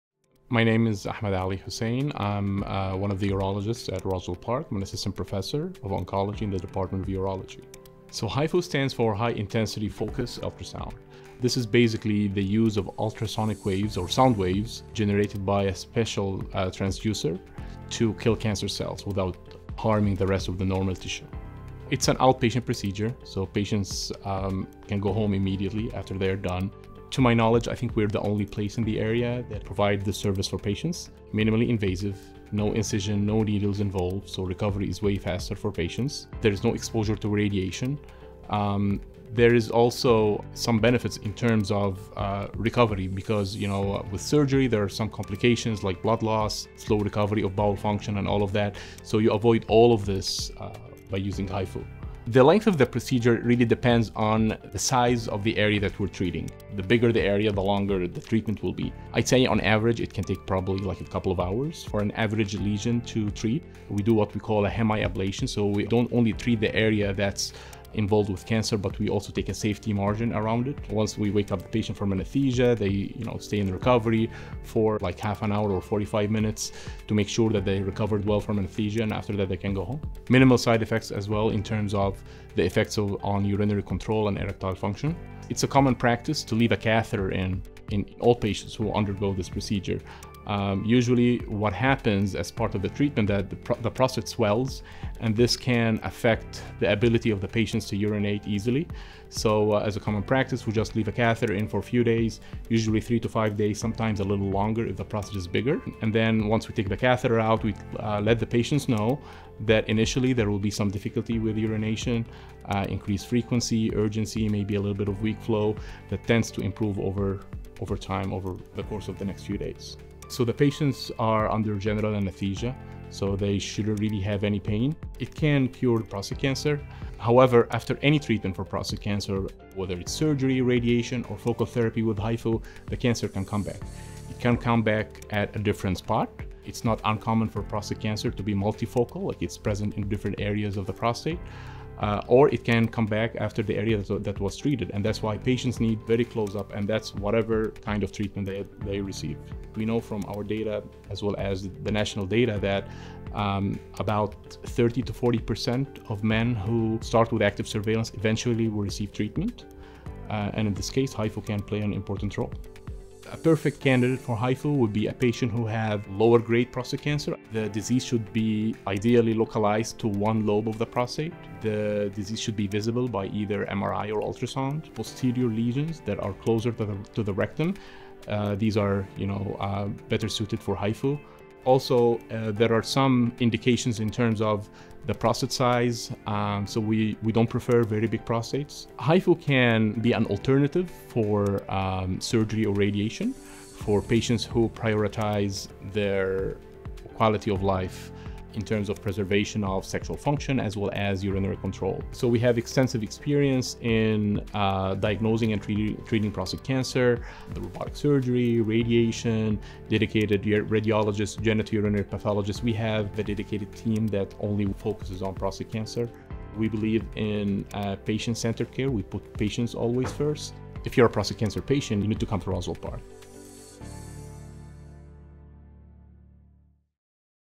HIFU Treatment for Prostate Cancer | Dr. Ahmed Aly Hussein
A new treatment for prostate cancer, high-intensity focused ultrasound (HIFU) is a minimally invasive, nonsurgical option that allows patients to go home the same day — HIFU uses highly targeted ultrasound waves to ablate or destroy cancer cells in the prostate.

Roswell Park is the only provider in Western New York to offer HIFU therapy for prostate cancer.